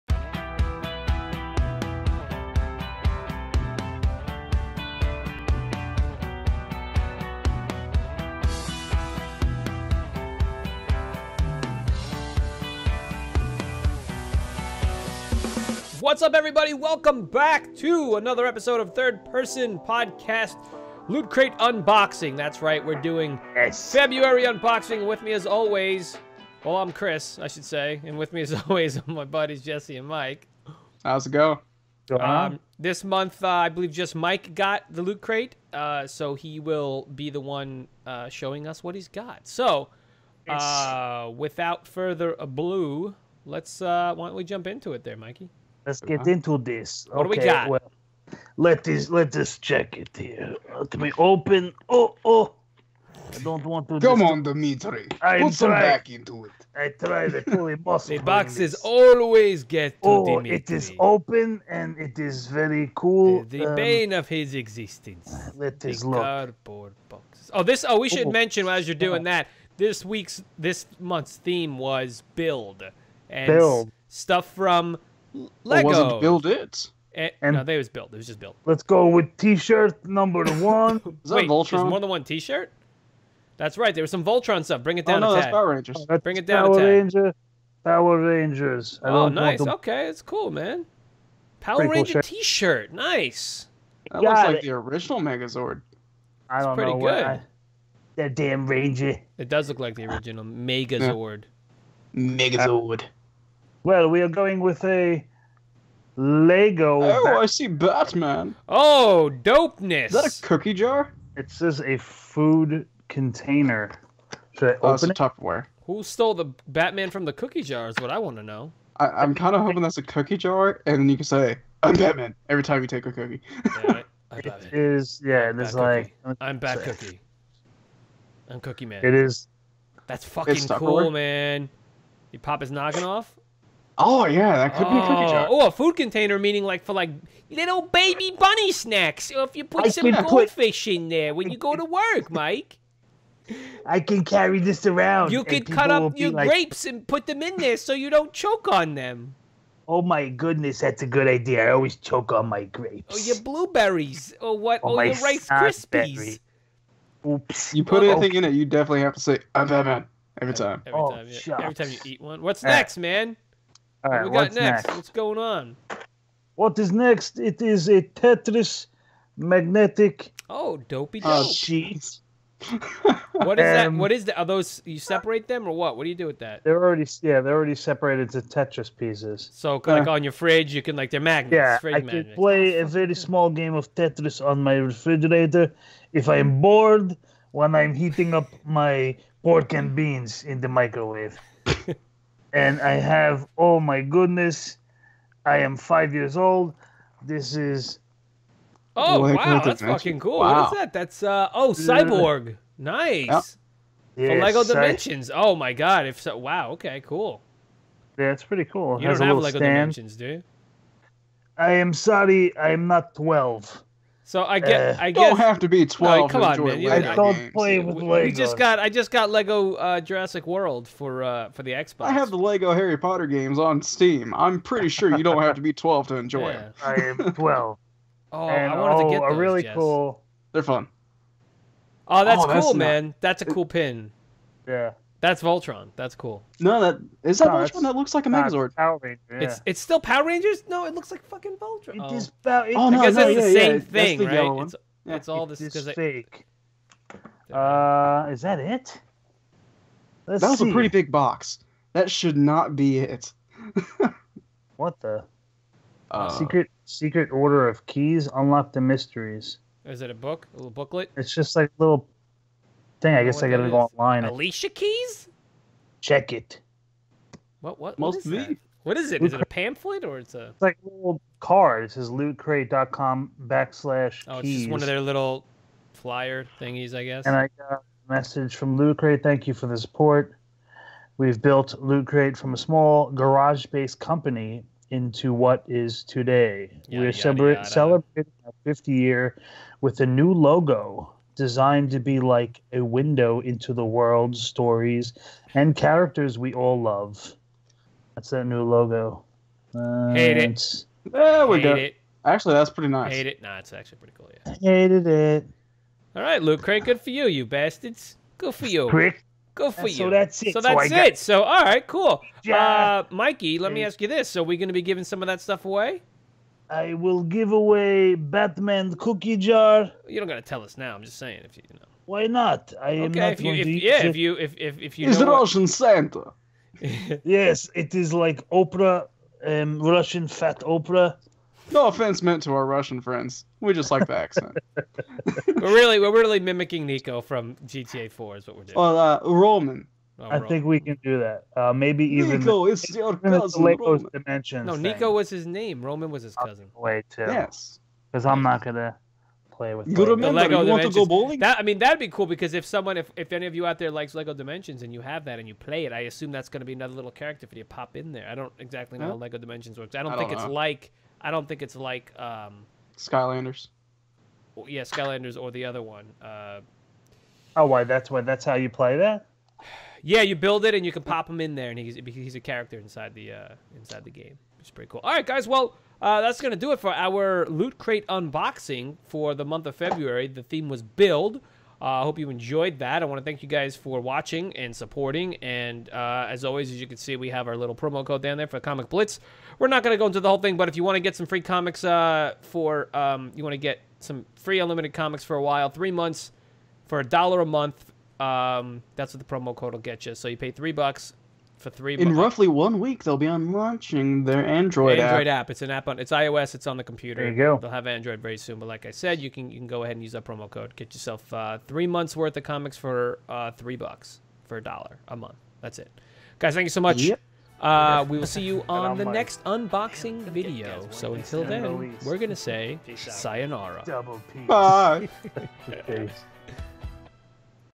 Lootcrate Unbox FEBRUARY 2017 - BUILD
This month's theme is "Build", today we are joined by Niko, Nikolai, and Roman. The guys have fun with the unboxing and even throw on some rough yet funny accents while they're at it. Pop a squat and enjoy, or Roman will take you bowling.

Fancy some comedy?